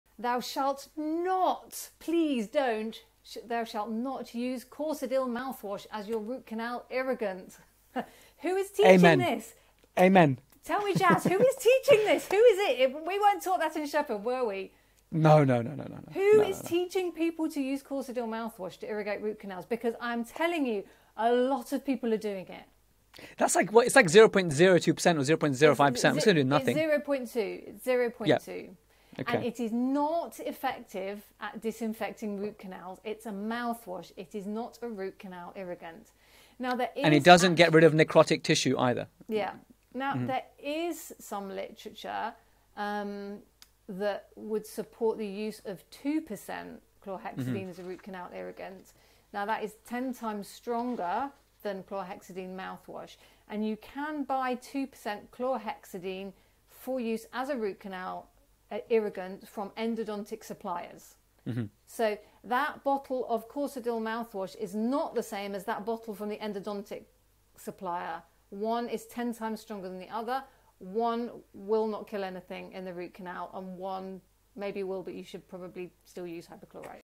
Don't use CHX for Root Canals!
STOP using Chlorhexidine Mouthwash for Root Canals ✋️

If you're going to use CHX then use 2% but if you think you will avoid the complications of a Hypochlorite accident, then think again! 😏

This was one of the 10 Commandments from PDP131 with @dr.lucynichols - which was your favourite one?

Listen to the full episode here: protrusive.co.uk/131

Check out all of the full episodes of the Protrusive Dental Podcast on protrusive.co.uk

-Jaz Gulati, Making it Tangible 👌